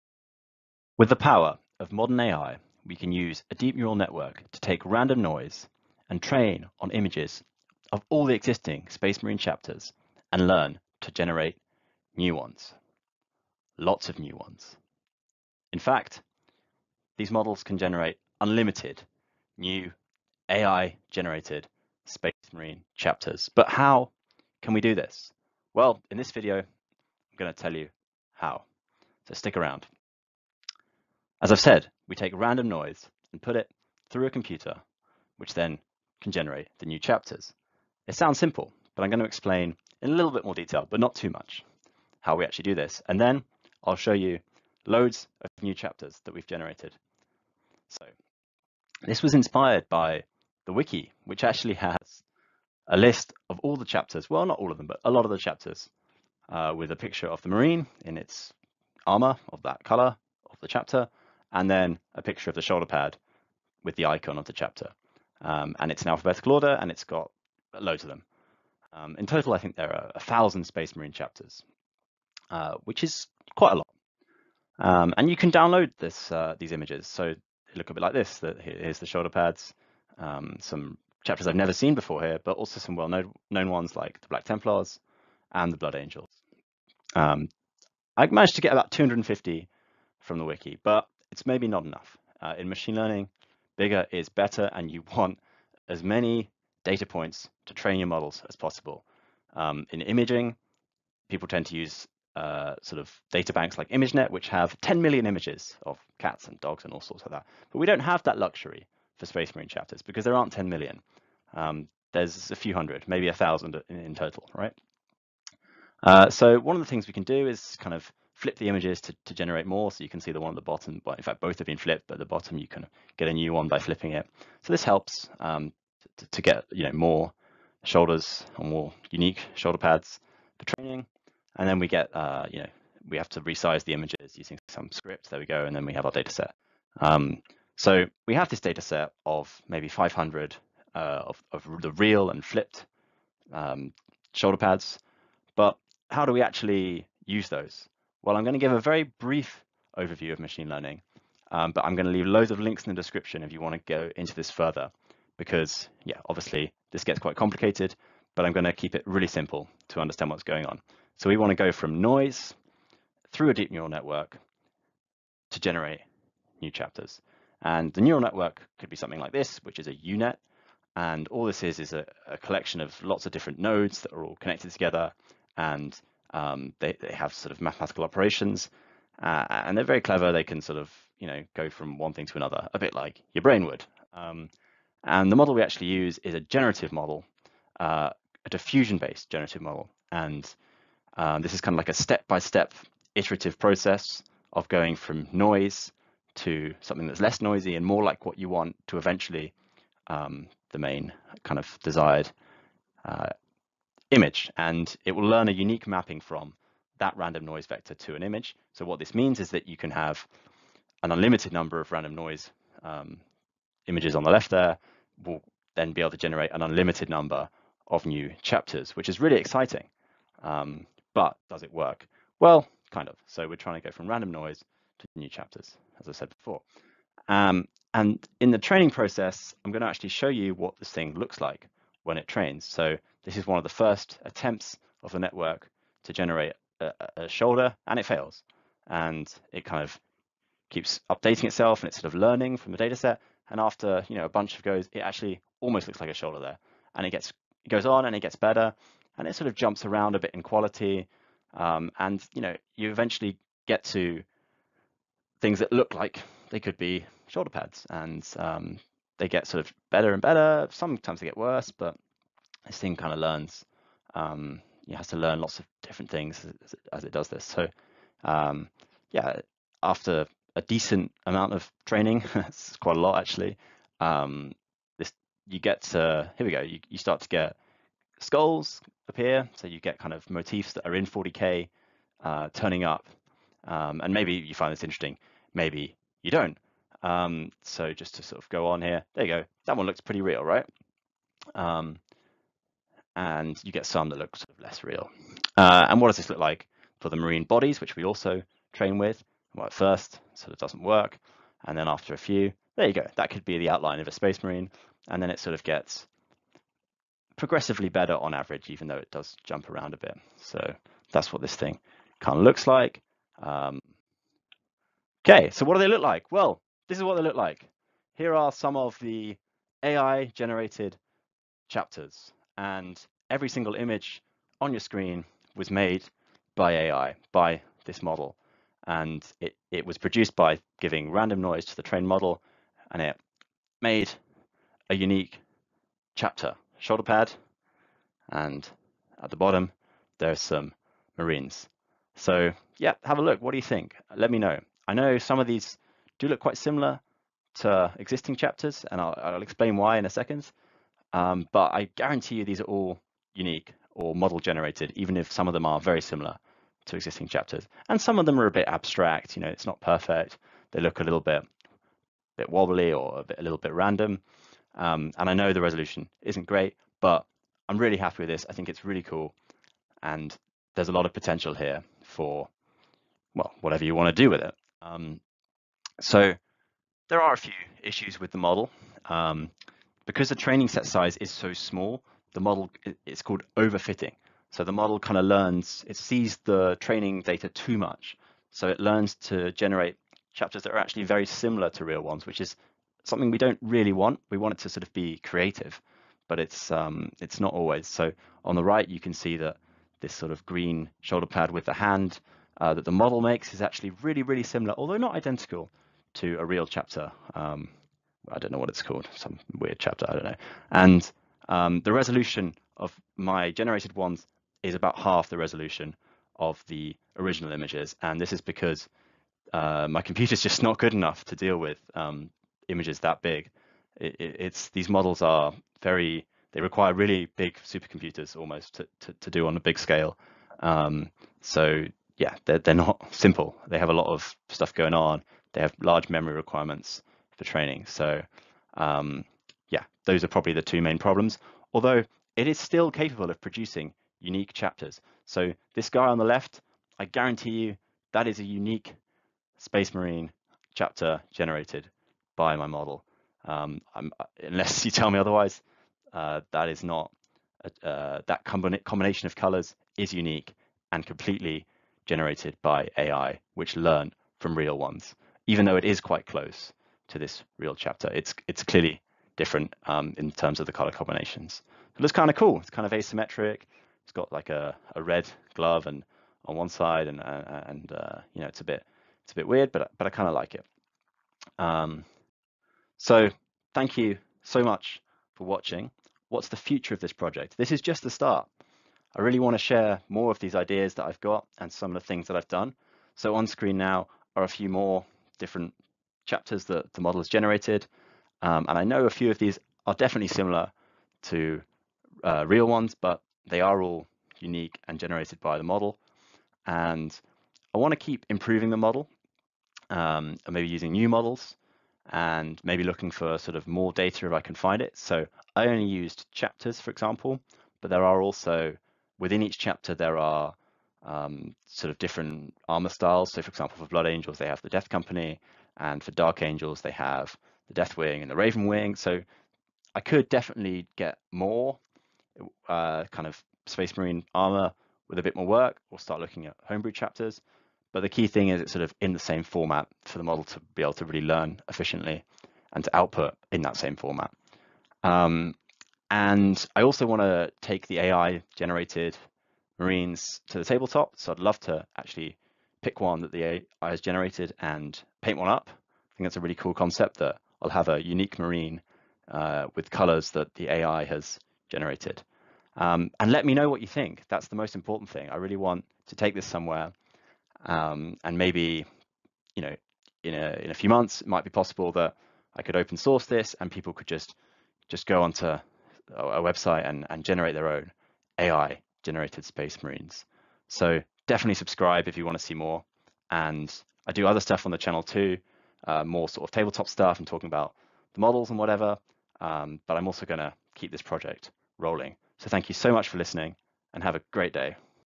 Using AI to Generate New Space Marine Chapters
Using machine learning I generate new chapters of Space Marines